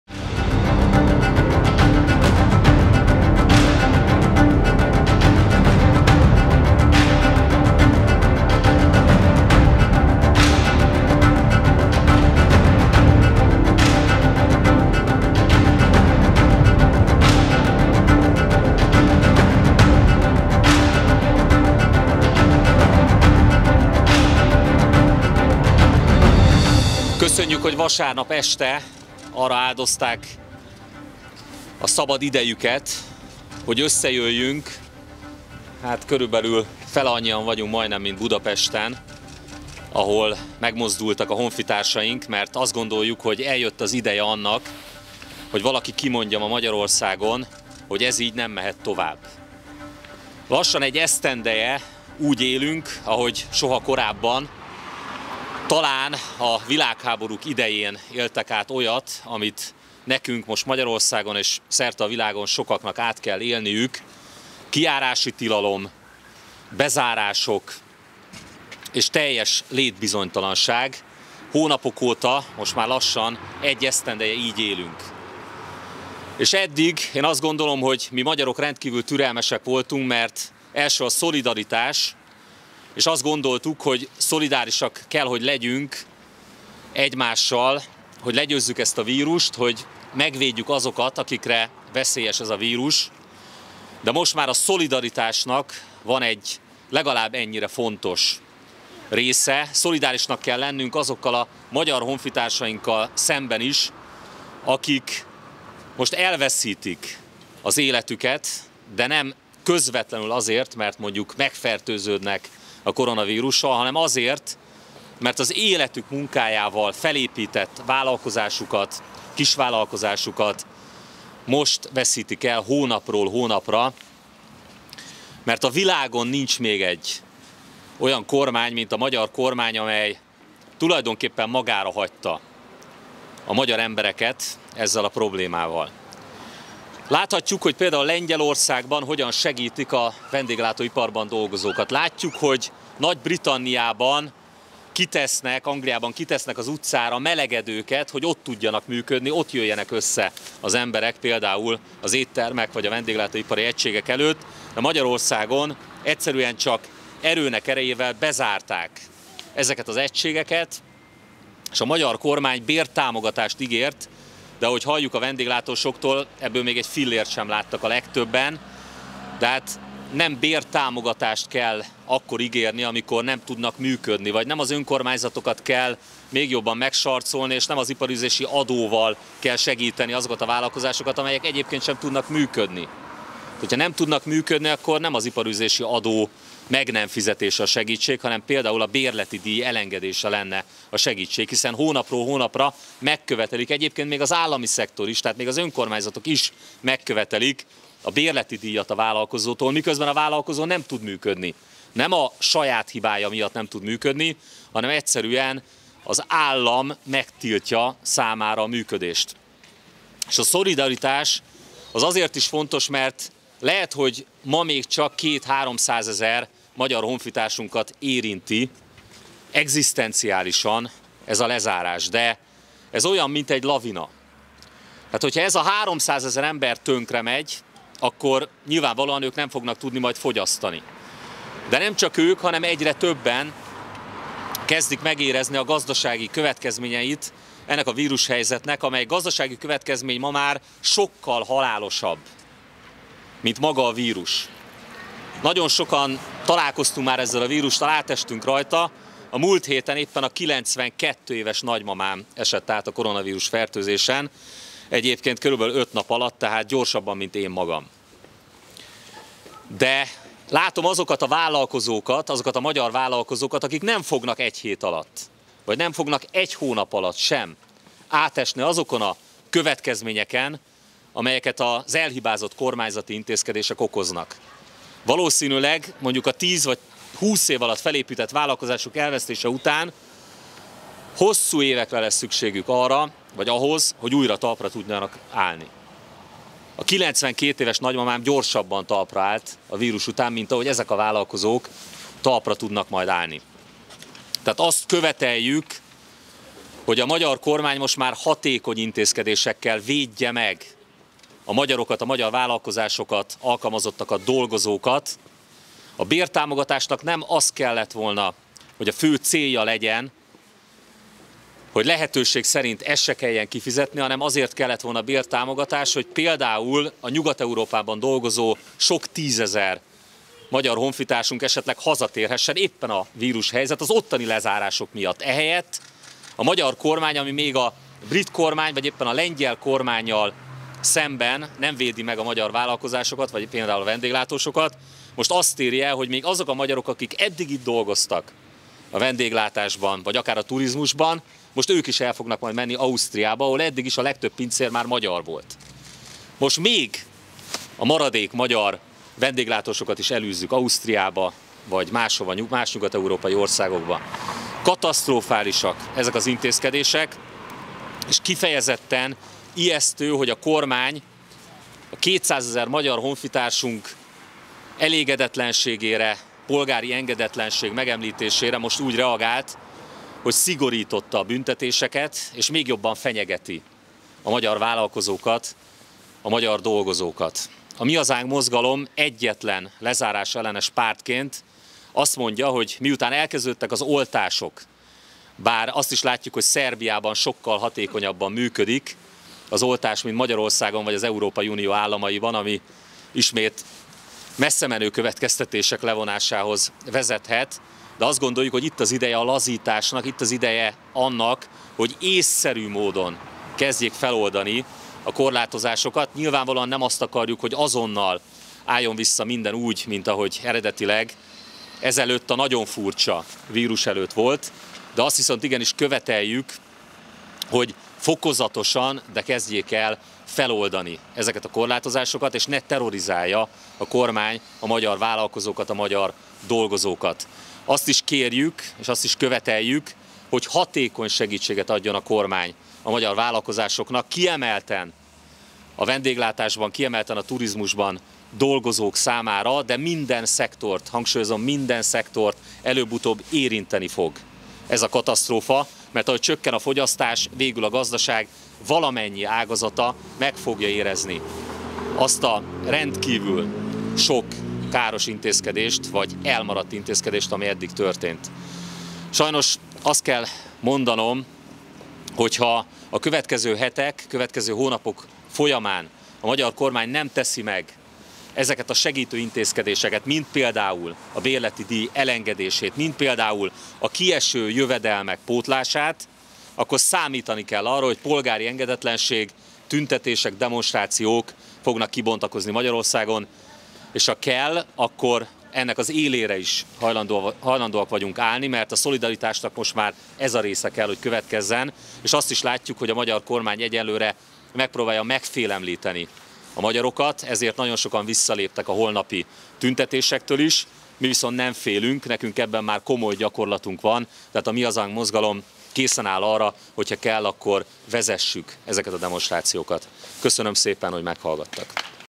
Elég a lezárásokból!
Tartják:
Toroczkai László, a Mi Hazánk elnöke, Csongrád-Csanád megyei képviselő
Laczkó Zsolt, Csongrád város képviselője
Dorogi György, Csongrád-Csanád megyei képviselő